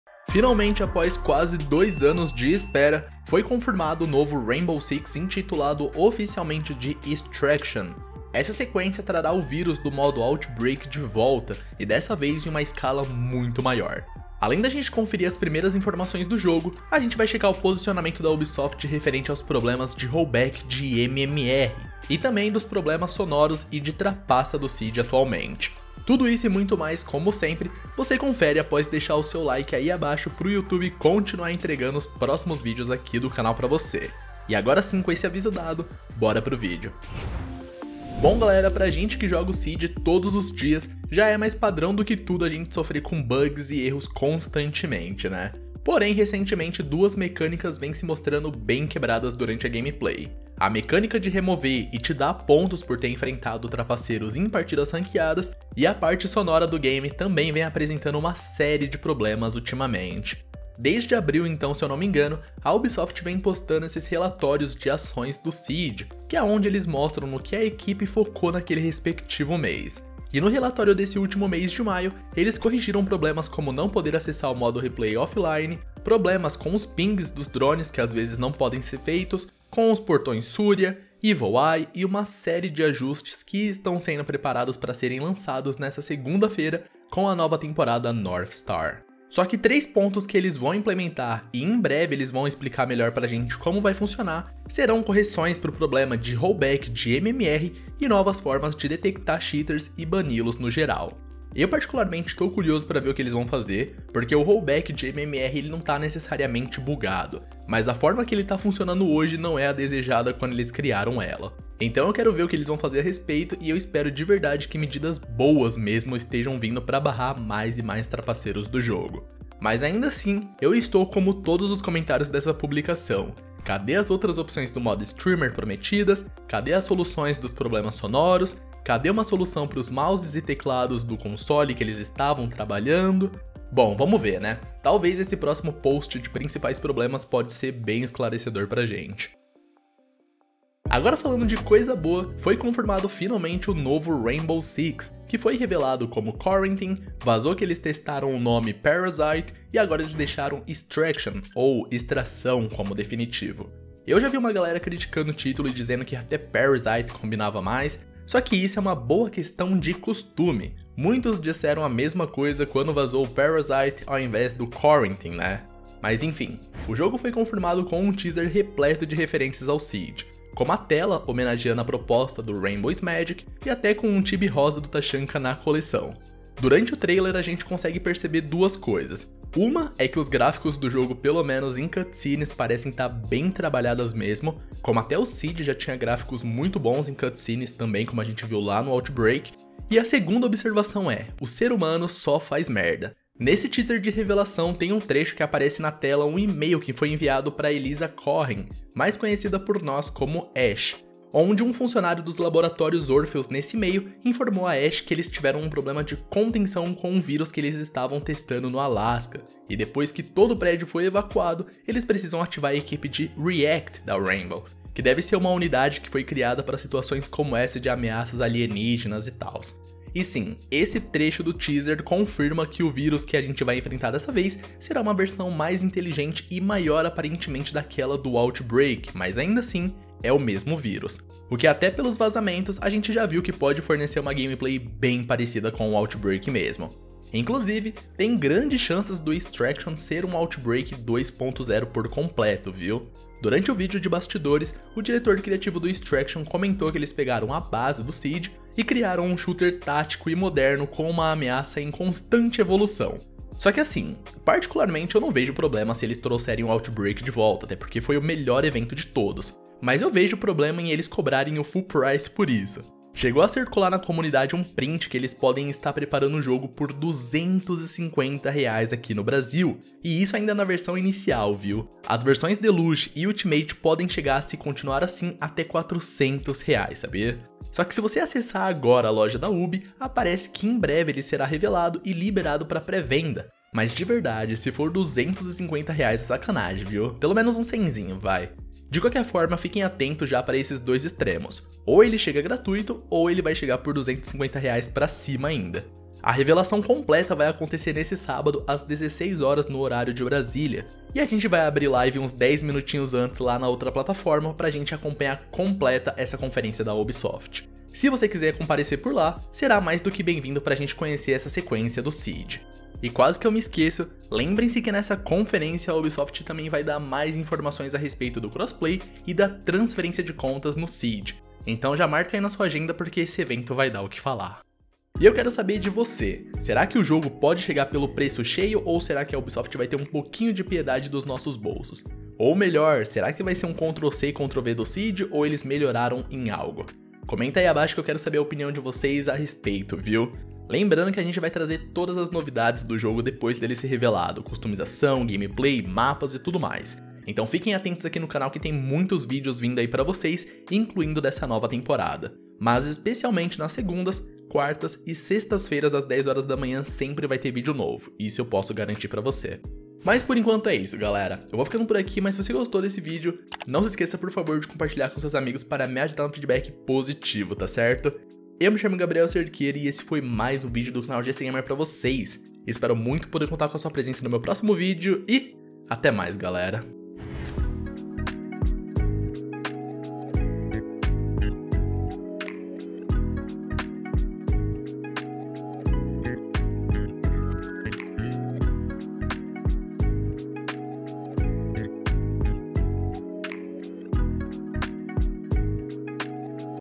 NOVO RAINBOW SIX: EXTRACTION CONFIRMADO, E LÁ VEM PANCADA! - Rainbow Six: Extraction
Finalmente após quase dois anos de espera, foi confirmado o novo Rainbow Six intitulado oficialmente de Extraction. Essa sequência trará o vírus do modo Outbreak de volta, e dessa vez em uma escala muito maior! Além da gente conferir as primeiras informações do jogo, a gente também vai checar o posicionamento da Ubisoft, referente aos problemas de rollback de MMR, e também dos problemas sonoros do Siege atualmente.

► Capítulos
00:00 Introdução
00:38 1. Problemas do Siege
02:21 2. Rainbow Six: Extraction
05:25 Fechamento